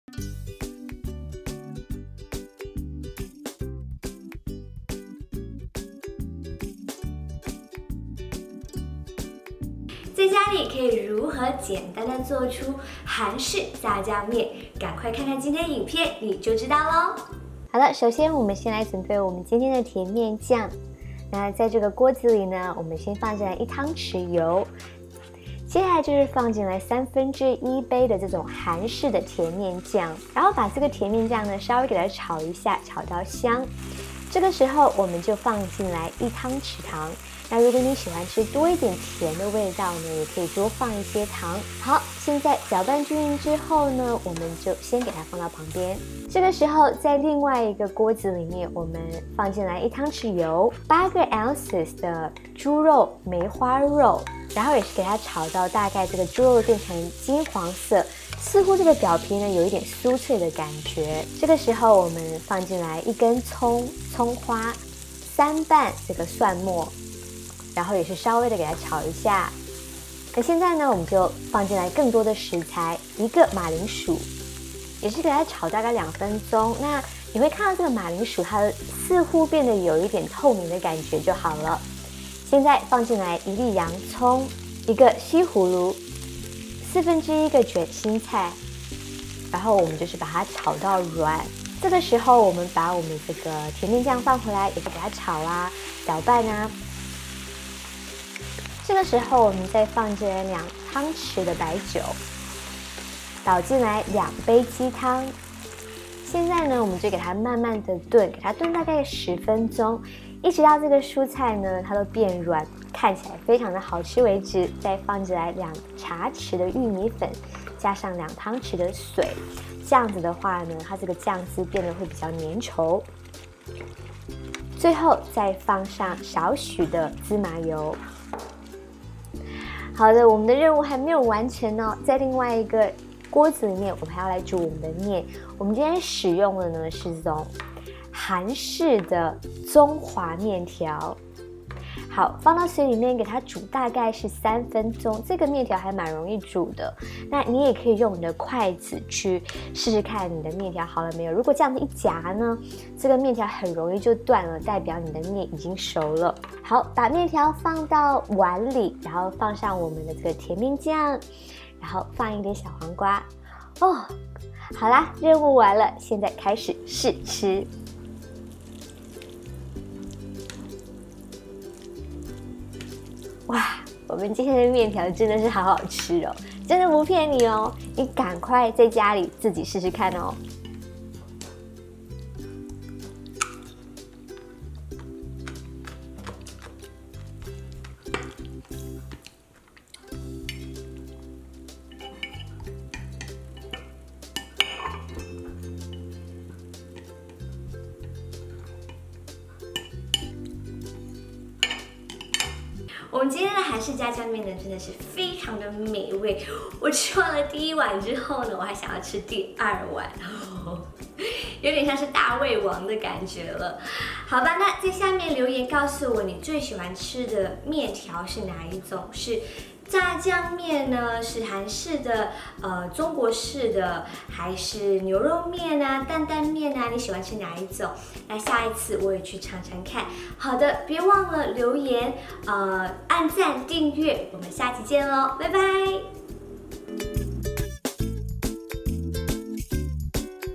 韓式必吃炸醬麵的家庭做法【美食天堂 CiCi's Food Paradise】
怎樣在家裡做出超簡單美味的韓式炸醬麵？趕快來看看這個影片！我們炸醬麵真的超美味的，我想你應該也肚子餓了吧？在下面留言告訴我你最喜歡的麵條是哪一種呢？下次呢我也去嚐嚐看！

1湯匙油
8盎司梅花肉
1根蔥
3瓣蒜
1個馬鈴薯
1個洋蔥
1個西葫蘆
1/4捲心菜
2杯雞湯
2湯匙米酒
2茶匙玉米粉和2湯匙水
少許芝麻油
8盎司麵條
黃瓜
醃蘿蔔

甜麵醬：
1湯匙油
1/3杯甜麵醬（춘장）
1湯匙糖